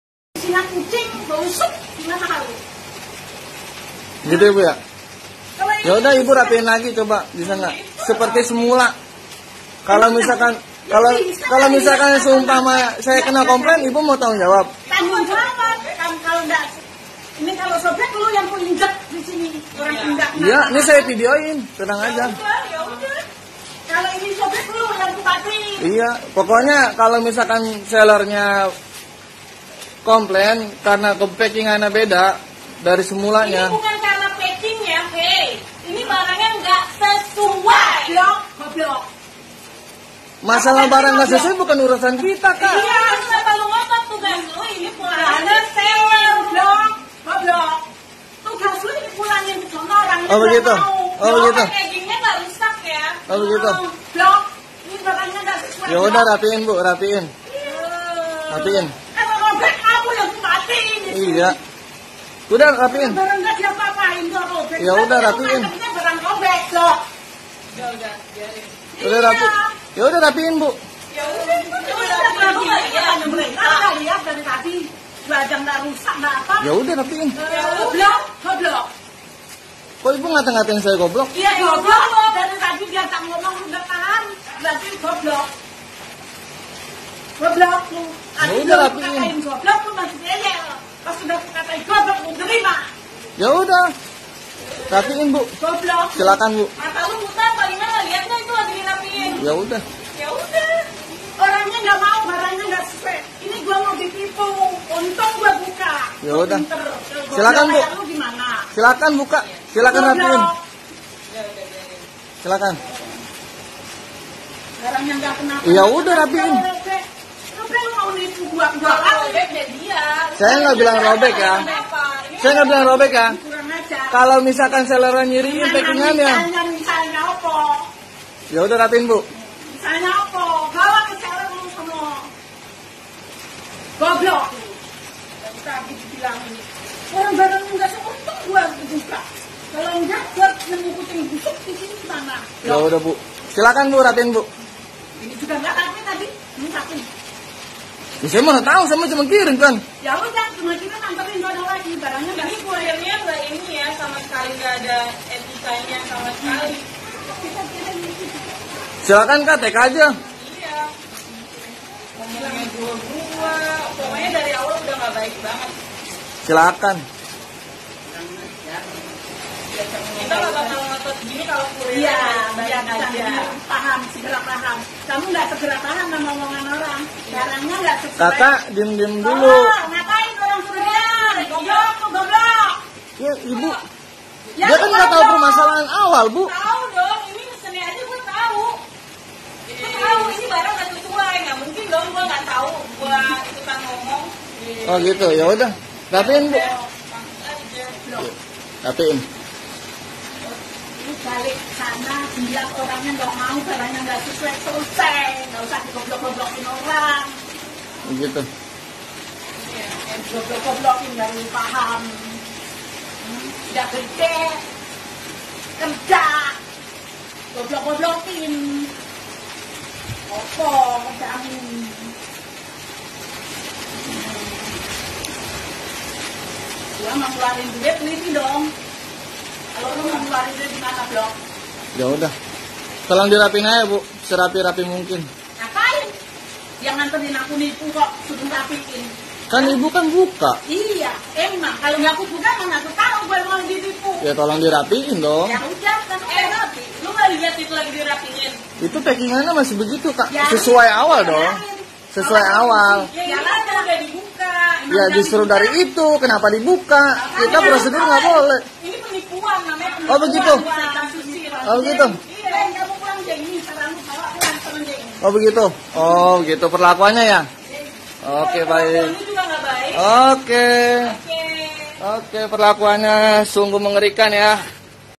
Viral!!! Emak-Emak Maki-Maki Kurir
Viral!!!

Hello guys ini lagi ada video viral yang lagi hangat-hangatnya, seorang emak-emak maki-maki kurir sampai dibilang bodoh. Selamat menyaksikan!